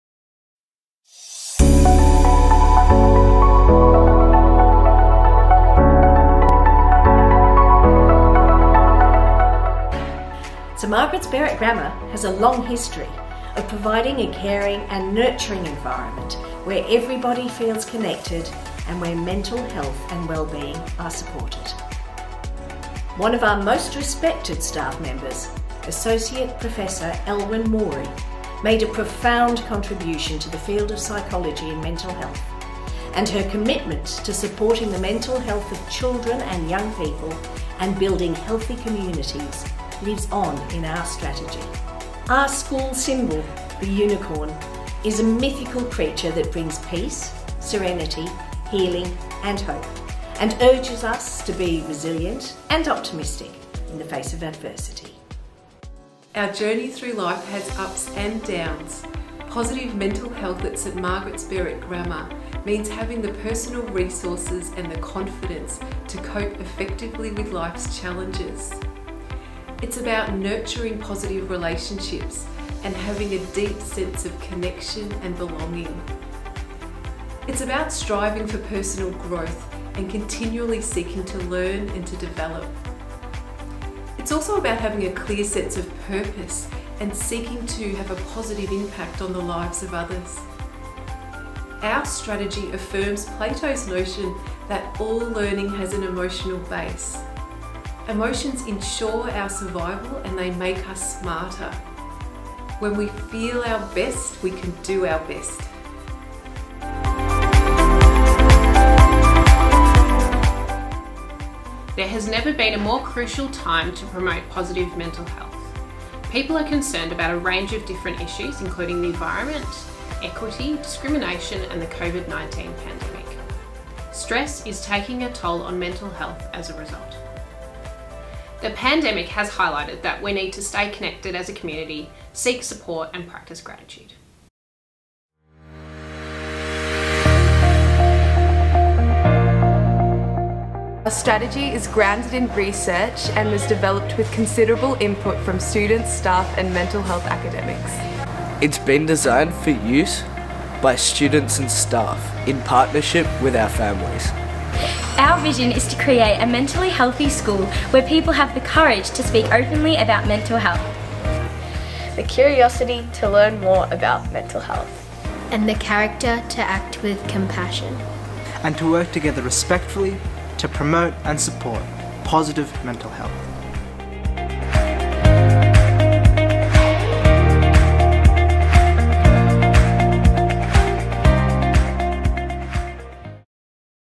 SMBG Positive Mental Health Strategy
We are pleased to share our new Positive Mental Health Strategy. Our strategy offers a clear vision for embedding principles of positive mental health across everything we do at St Margaret’s Berwick Grammar. We look forward to bringing our ideas to life and to continuing to work together to promote positive mental health and wellbeing.